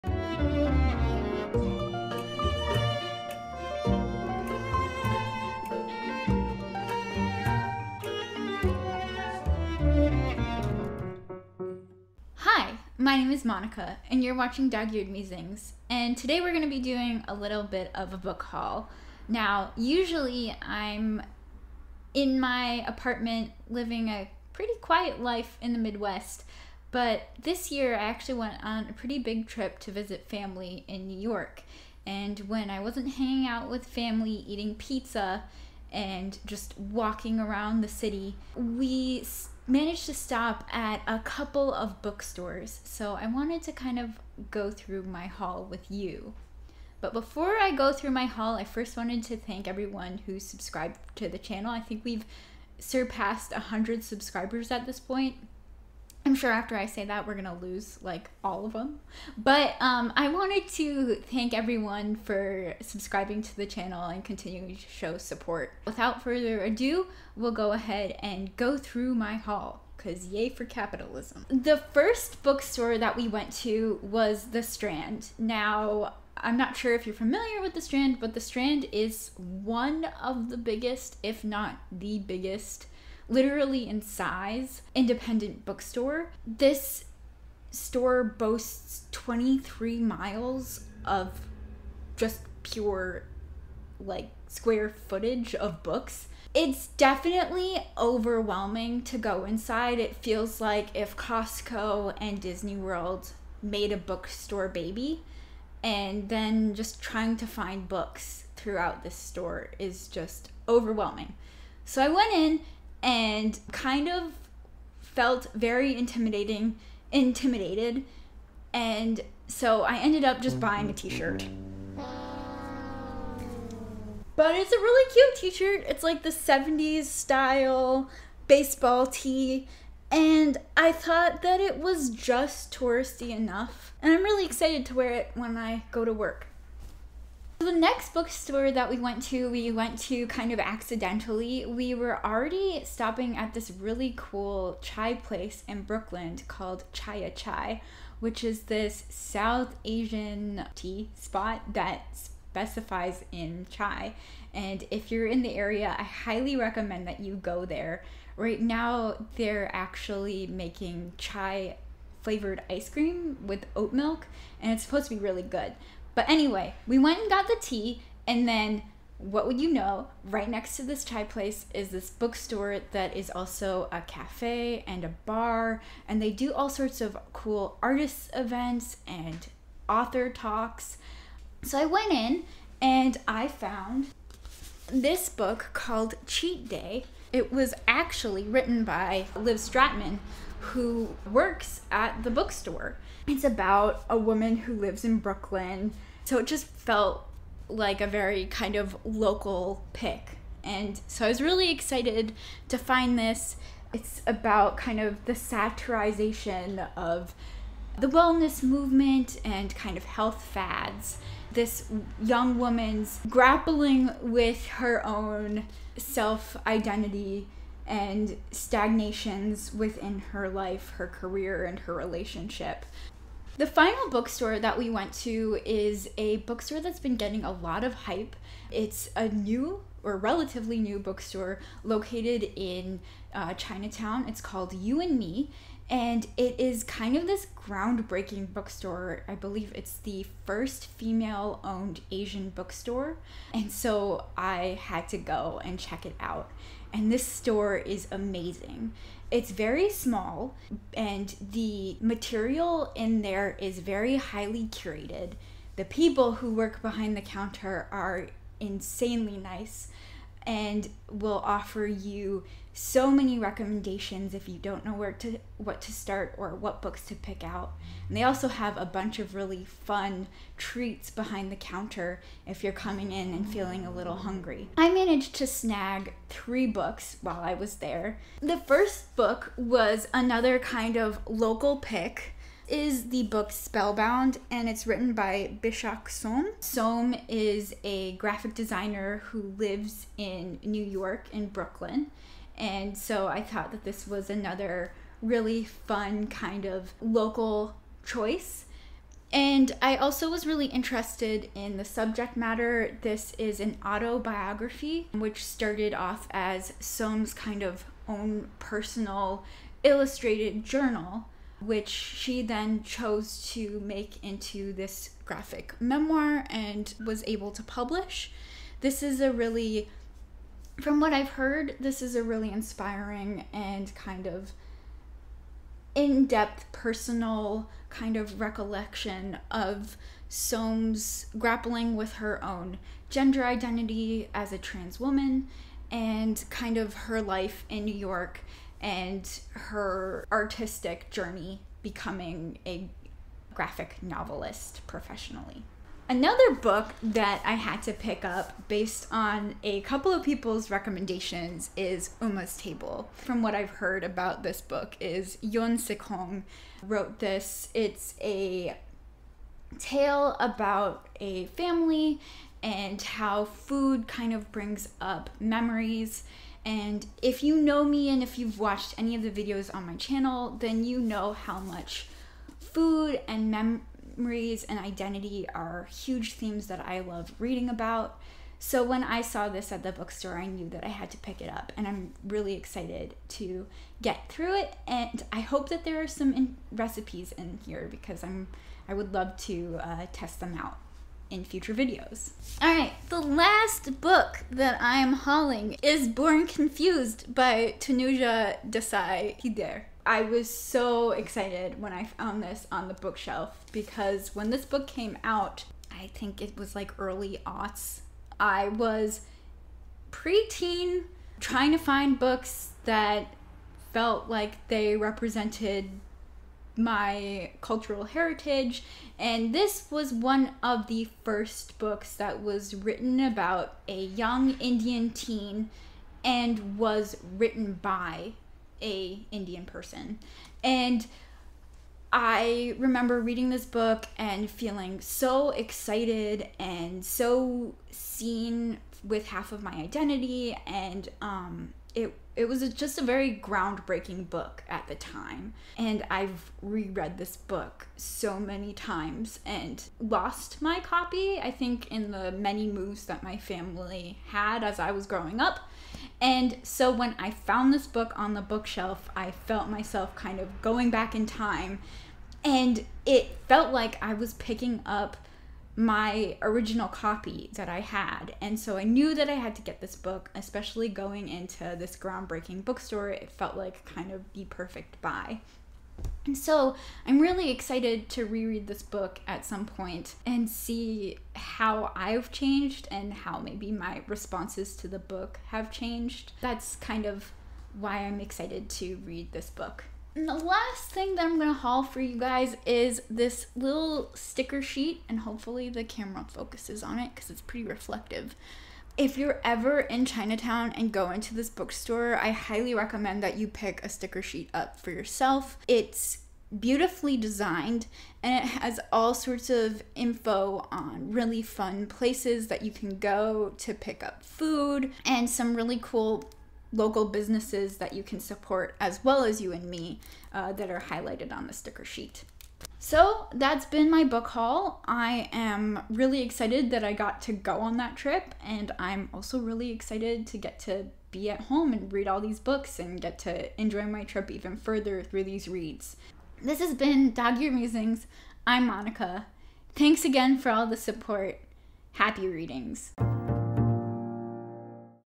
3 of the most interesting book stores in new york book haul
Hope you enjoy this short book haul from my trip to New York this summer!

Monika

Chapters
0:00 - Intro
1:11 - The Strand
2:17 -  Book Club
3:42 - Yu & Me Books